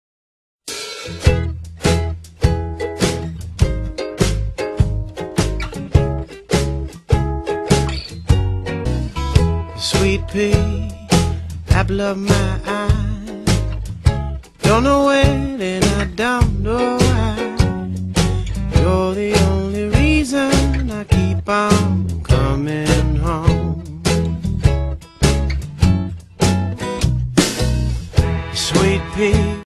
►The Duggar Family are Sweet Peas
I love the Duggars so much. I found all these pictures on their All new photos. Well most of them are new to me. They are such adorable little kids. I love them. Please coment and thumbs up? Thanks :D